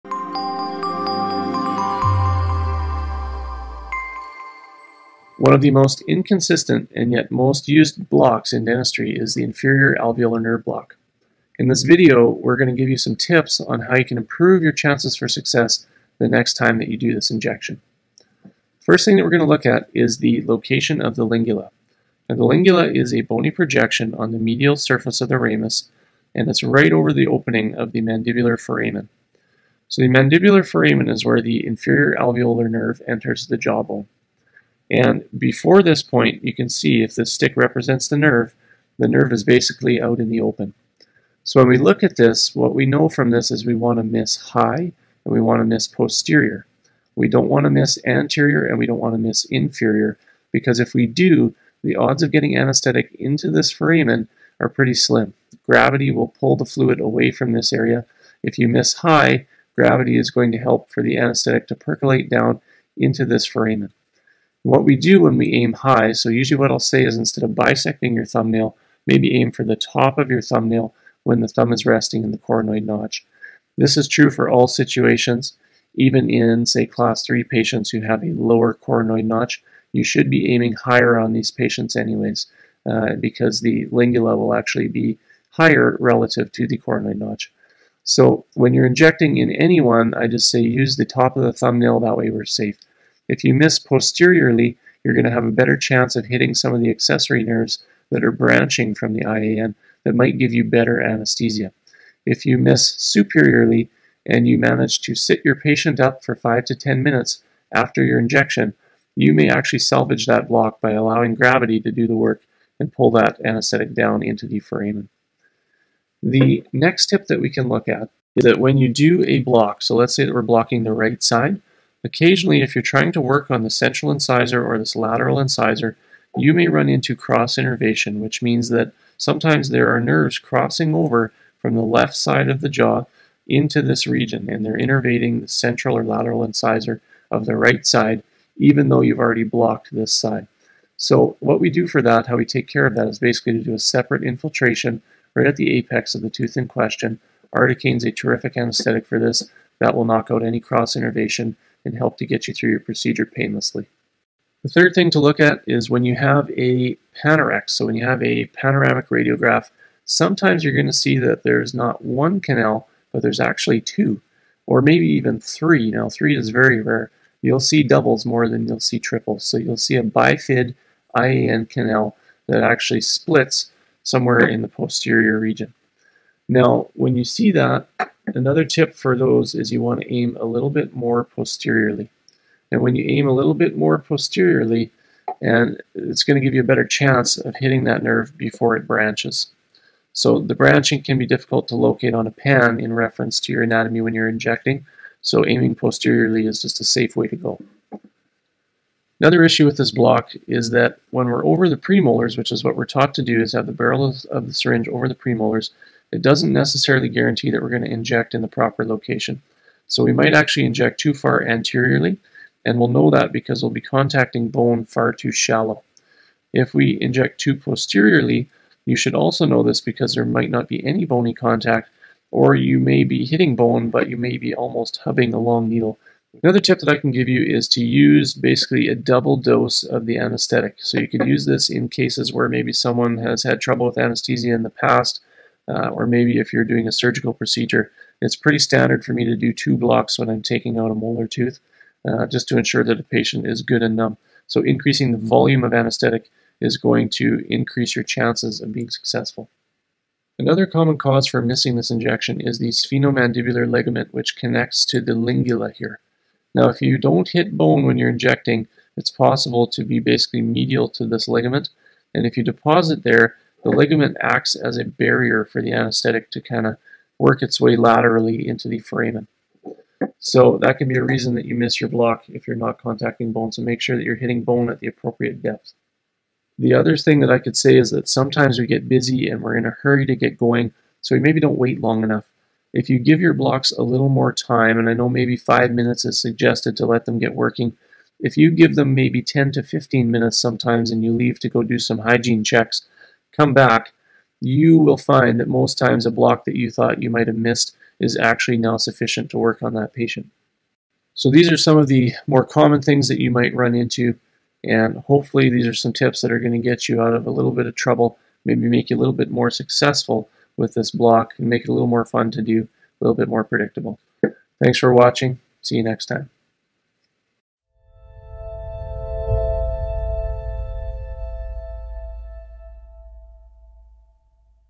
Inferior Alveolar Nerve Block Reasons For Failure | Local-Anesthetic.com
The most utilized injection in dentistry is also an injection with a significant failure rate.  Discover the reasons why you may be missing your inferior alveolar nerve blocks and see possible solutions to overcome these failures.